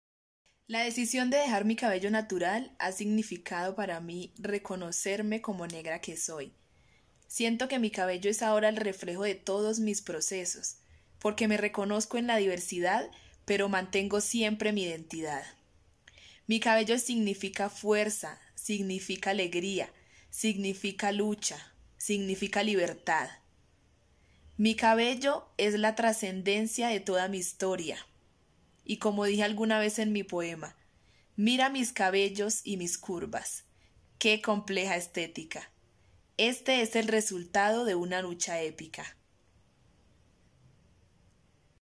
EPT: Angie González Qué Significa tu Cabello para ti?
Angie nos cuenta que significa en este momento su cabello para ella.